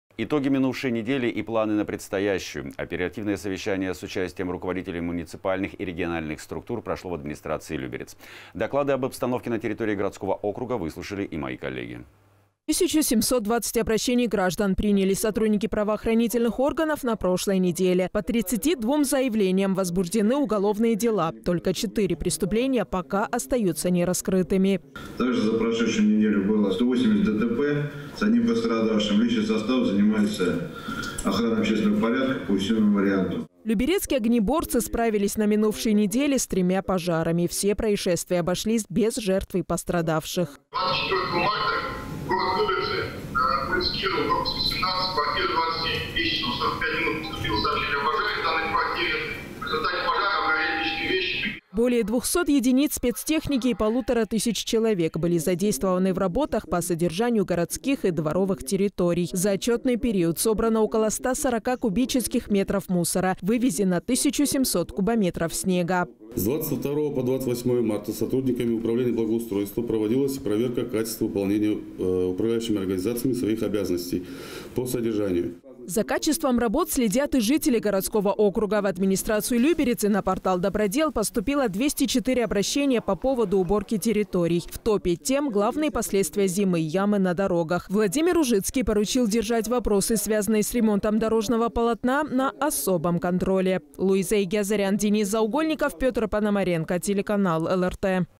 Еженедельное совещание в администрации
Итоги минувшей недели и планы на предстоящую. Оперативное совещание с участием руководителей муниципальных и региональных структур прошло в администрации Люберец. Доклады об обстановке на территории городского округа выслушали мои коллеги.

1 720 обращений граждан приняли сотрудники правоохранительных органов на прошлой неделе. По 32 заявлениям возбуждены уголовные дела. Только 4 преступления пока остаются нераскрытыми.

Евгений Романцев, начальник МУ МВД России «Люберецкое»:
«Также за прошедшую неделю было 180 ДТП с одним пострадавшим. Личный состав занимается охраной общественного порядка по усиленному варианту».

Люберецкие огнеборцы справились на минувшей неделе с тремя пожарами. Все происшествия обошлись без жертв и пострадавших.

Максим Катаев, заместитель начальника люберецкого пожарно-спасательного гарнизона Московской области:
«24 марта, город Люберцы, ул. Кирова, дом 17, кв. 27. В 3:45 поступило сообщение о пожаре в данной квартире. В результате пожара обгорели личные вещи, предметы мебели».

Более 200 единиц спецтехники и полутора тысяч человек были задействованы в работах по содержанию городских и дворовых территорий. За отчетный период собрано около 140 кубических метров мусора, вывезено 1 700 кубометров снега.

СНХ 1409 ТИТР: Залимхан Гаджиев, заместитель главы администрации г.о. Люберцы:
«С 22 по 28 марта сотрудниками управления благоустройства проводилась проверка качества выполнения управляющими организациями своих обязанностей по содержанию».

За качеством работ следят и жители городского округа. В администрацию Люберец и на портал «Добродел» поступило 204 обращения по поводу уборки территорий. В топе тем главные последствия зимы – ямы на дорогах. Владимир Ружицкий поручил держать вопросы, связанные с ремонтом дорожного полотна, на особом контроле.

Луиза Егиазарян. Денис Заугольников. Петр Пономаренко.